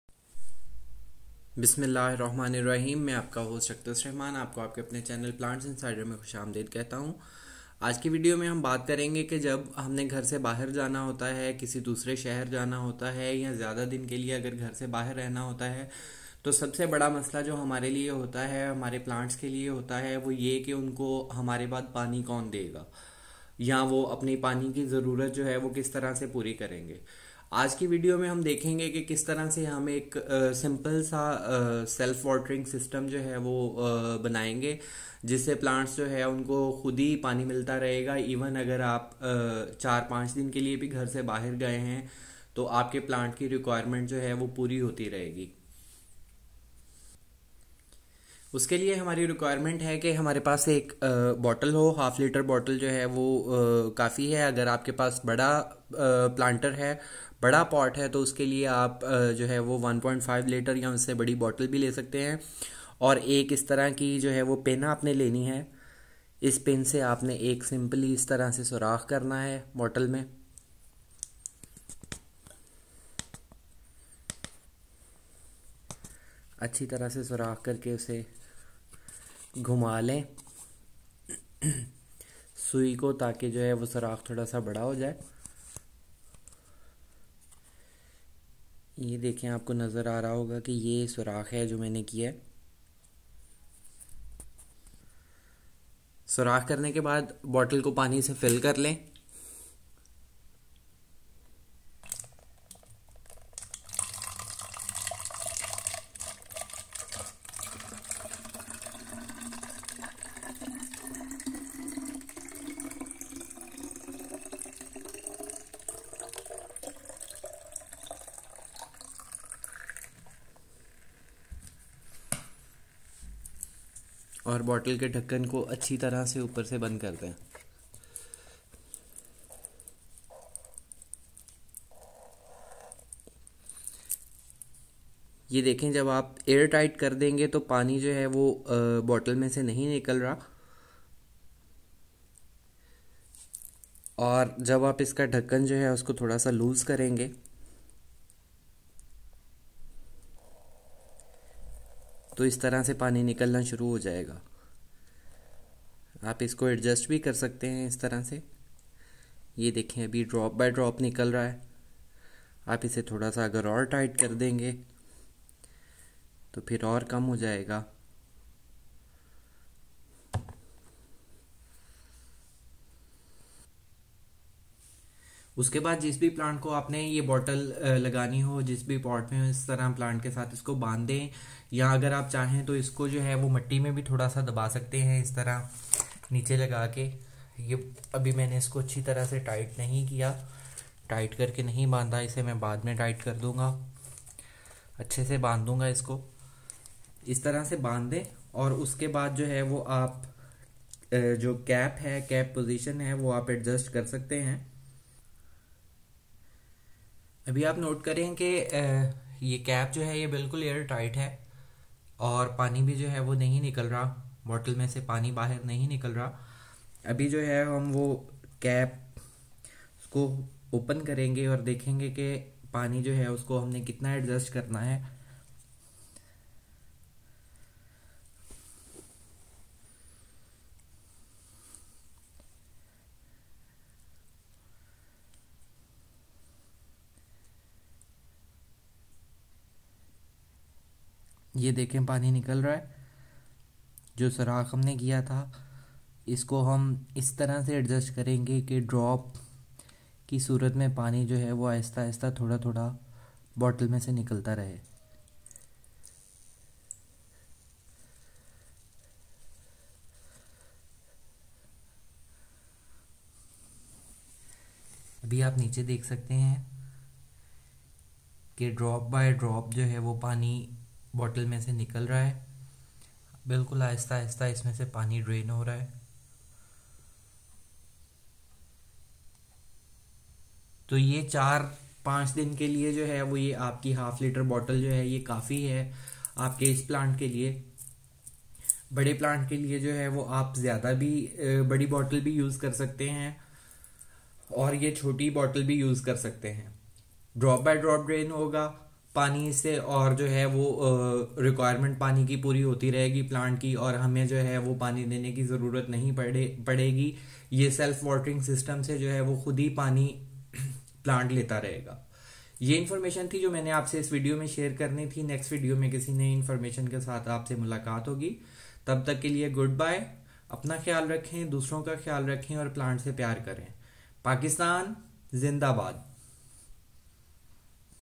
Make Self Watering System for Plants in Pots
► Make Self Watering System for Plants in Pots, This is a plant watering system that we can make at home which automatically irrigates plants when we are at vacations, this is the cheapest and easy automatic plant watering system.
Simple self watering system for plants and how we can use plastic bottles for our plants watering, Don't worry about your plants when you are going for vacations, your plants have an excellent system to fulfill watering requirements, Keep Watching!